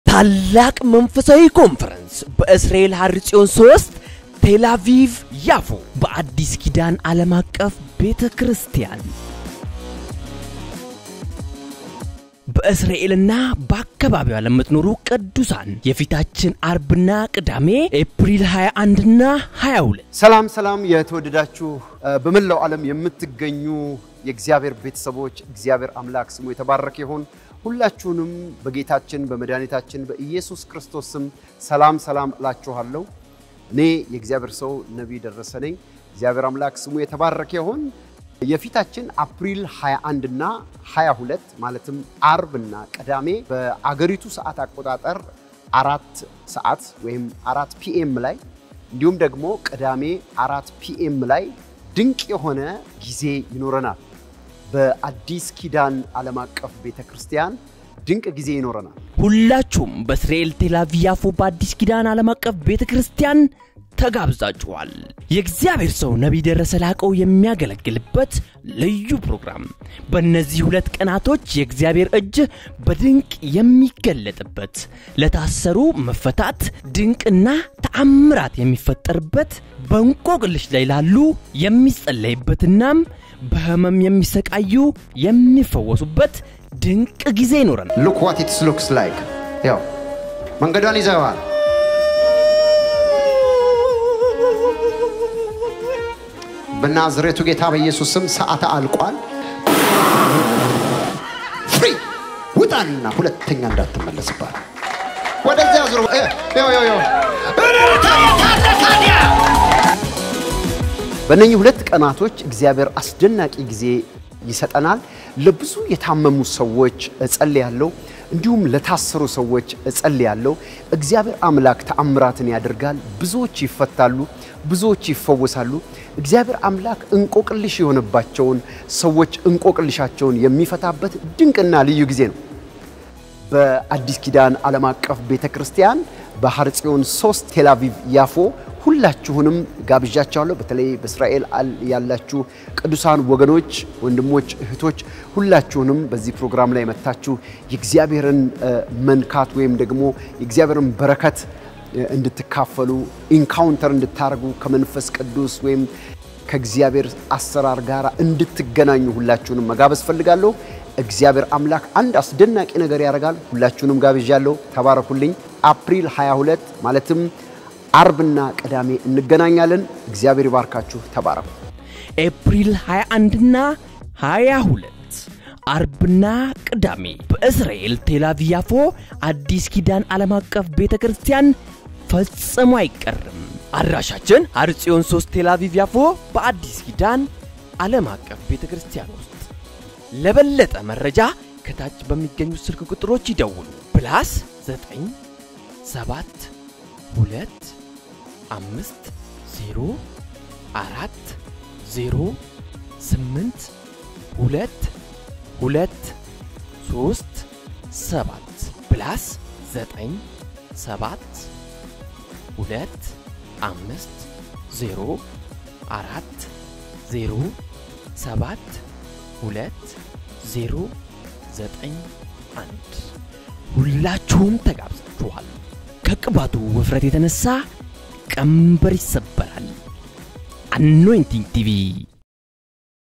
🇮🇱 እስራኤል_ቴላቪቭ Isreal_Tel_Aviv ታላቅ መንፈሳዊ ኮንፈረንስ 📆 April 21 እና April 22 በአዲስ ኪዳን ዓለም አቀፍ ቤተክርስቲያን
ታላቅ መንፈሳዊ ኮንፈረንስ በእስራኤል ሀርፂወን 3 ቴላቪቭ ያፎ ,በአዲስ ኪዳን ዓለም አቀፍ ቤተክርስቲያን
ከነቢይ ደረሰ ላቀው ጋር ለብዙዎች የሚጸለይበት ብዙዎች የሚፈወሱበት ከአጋንንት እስራት የሚፈቱበት ድንቅ ጊዜ
🇮🇱 እስራኤል_ቴላቪቭ Isreal_Tel_Aviv ታላቅ መንፈሳዊ ኮንፈረንስ በእስራኤል ሀርፂወን 3 ቴላቪቭ ያፎ 🇮🇱 📆 April 21 እና April 22 ,በአዲስ ኪዳን ዓለም አቀፍ ቤተክርስቲያን
☎️ ለበለጠ መረጃ እነኚህን ስልኮች ይጠቀሙ  👇 👇 👇

ADDRESS
BISHOFTU-DEBREZEIT
/ETHIOPIA/